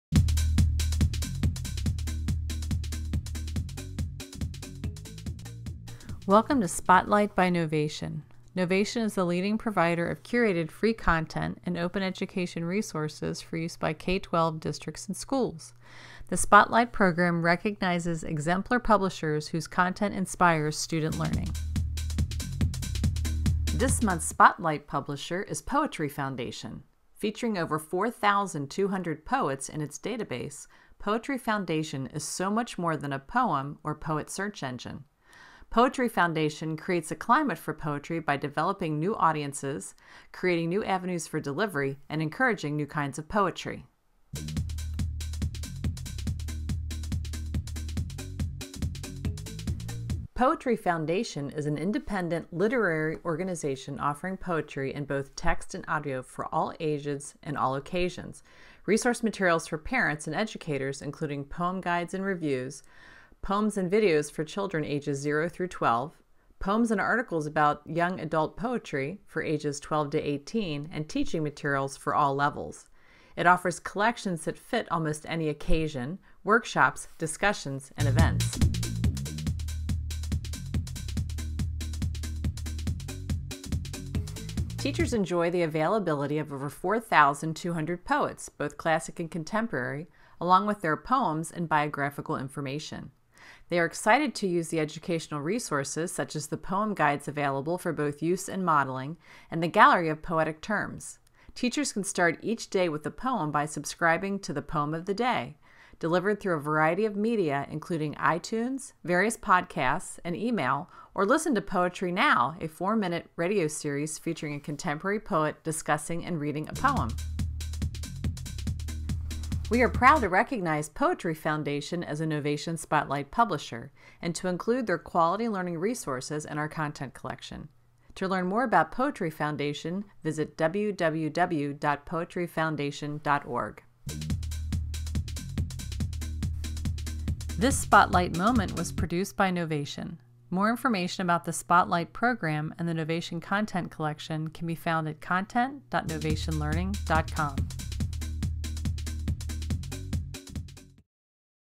Knovation Spotlight: Poetry Foundation (April 2018)
Knovation is proud to recognize Poetry Foundation as a Knovation Spotlight publisher and to include their quality learning resources in our Content Collection. To learn more about Poetry Foundationl, visit their